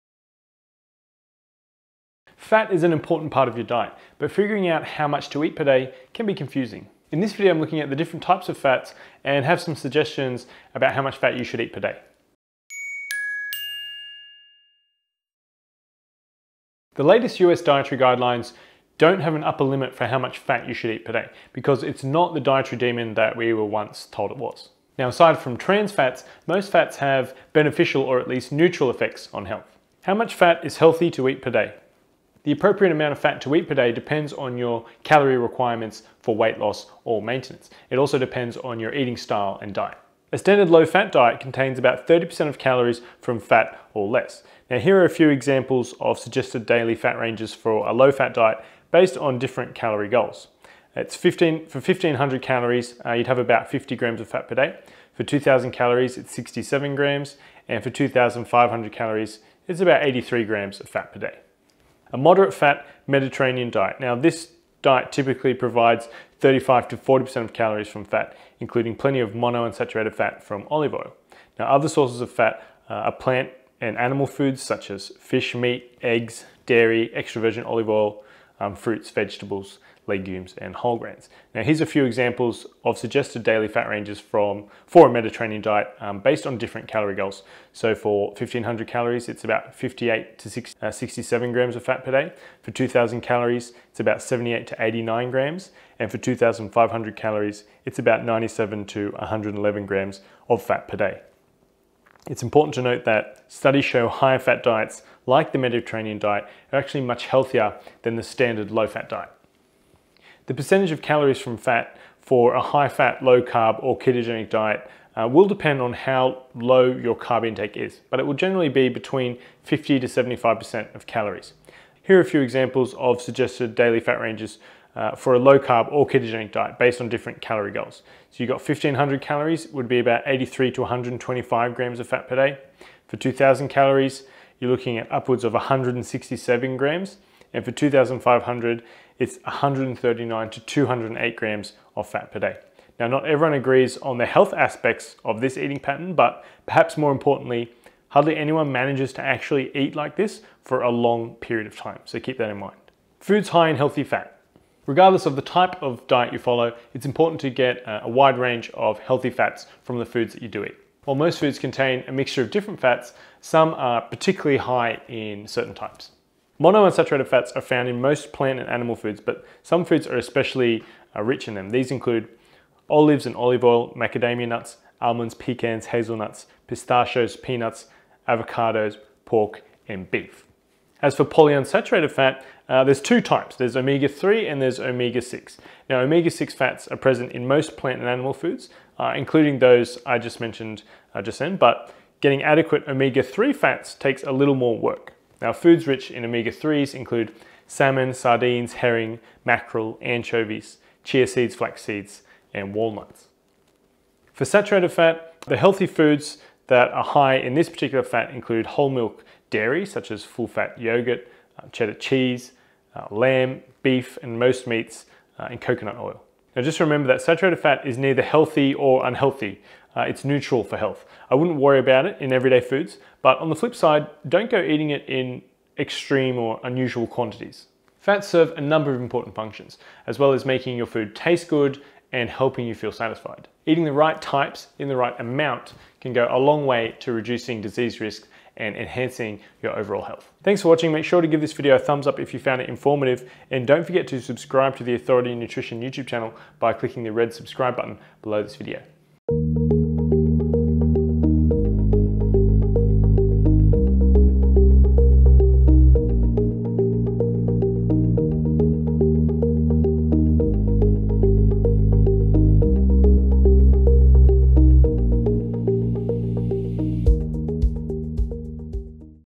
How Many Grams of Fat Should You Eat Per Day?
Fat is an important part of your diet, but figuring out how much to eat can be confusing. In this video I'm looking at the different types of fat and have some suggestions for how much to eat per day.